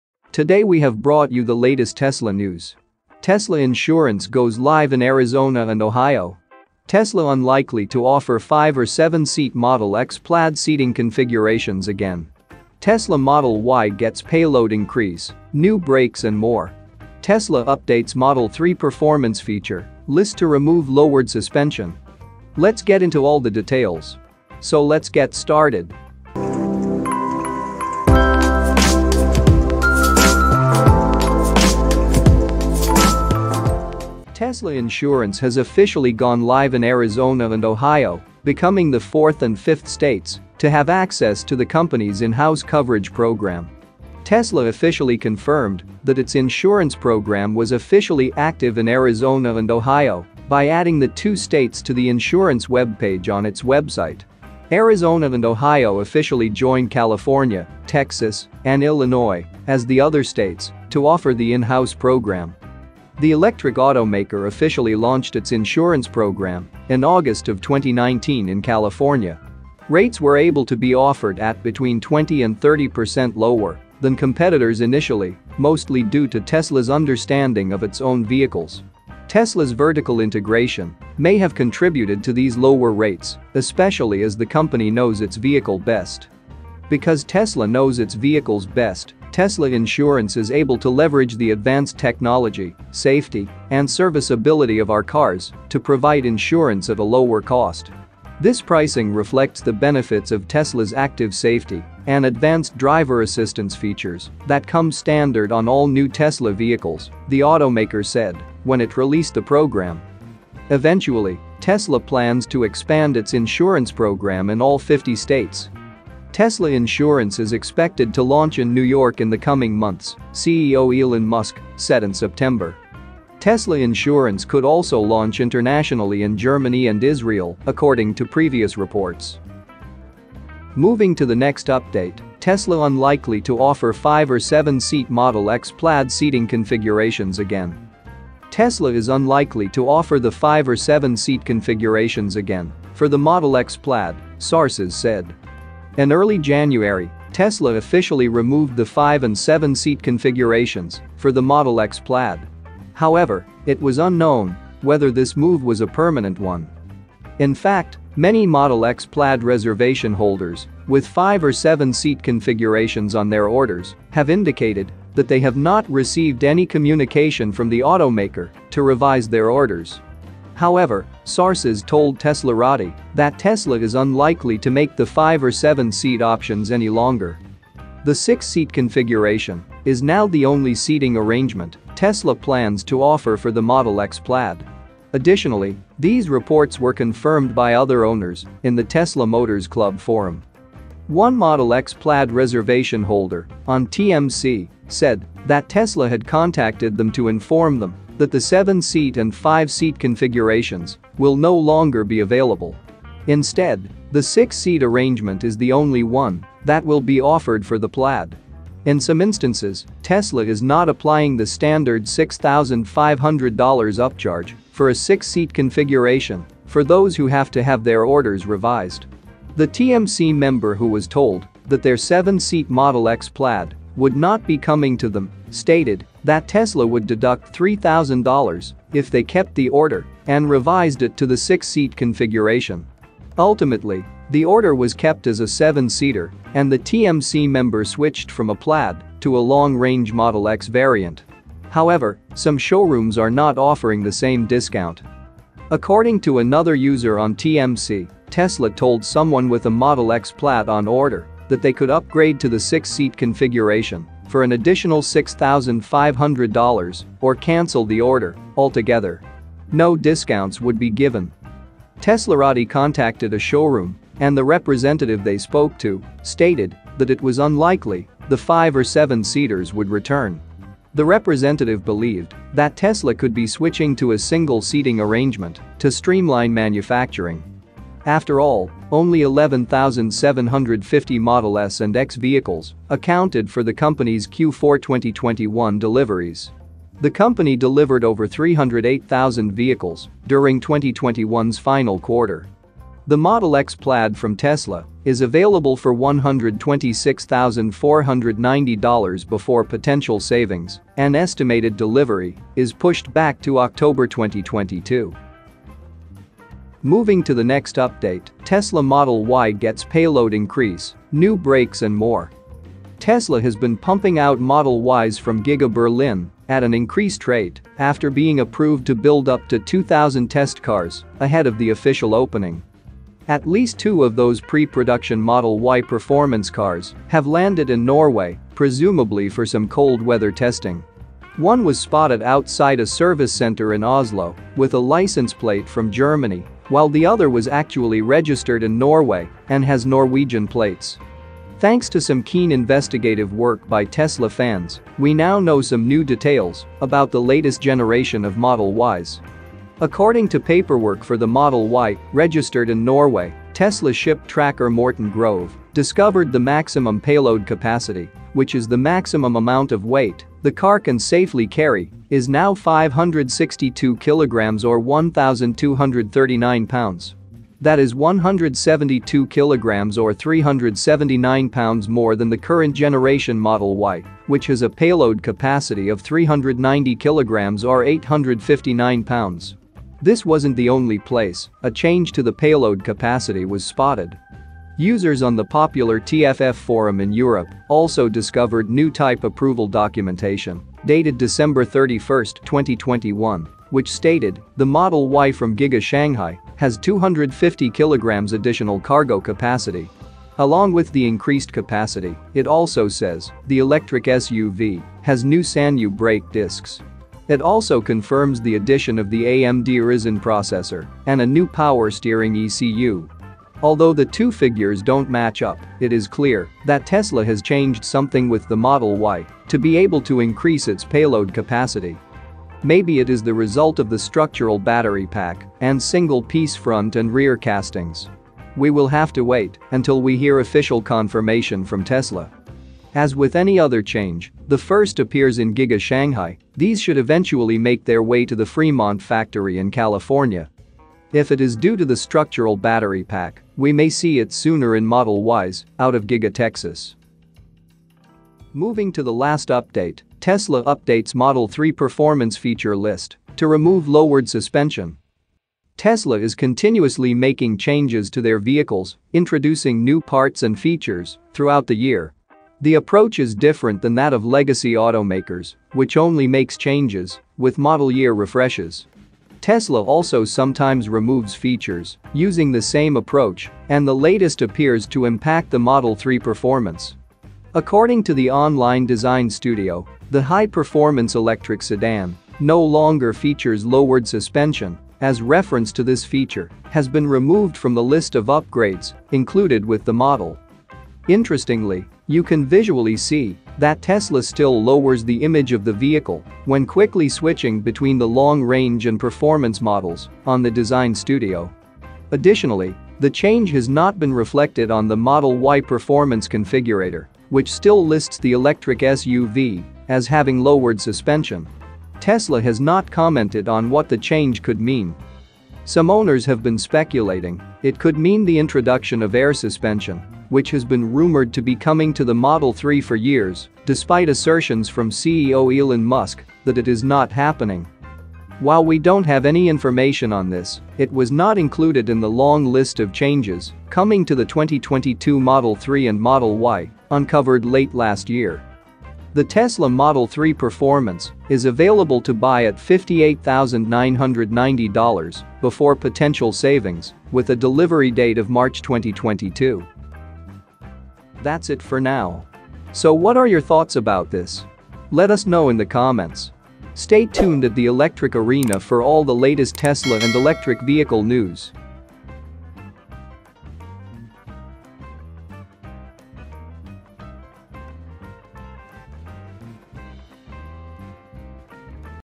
Tesla's New Model Y Gets More New Features & More Updates!
Tesla Insurance goes live in Arizona and Ohio
Tesla unlikely to offer five or seven-seat Model X Plaid seating configurations again
Tesla Model Y gets payload increase, new brakes and more
Tesla updates Model 3 Performance feature list to remove “lowered suspension”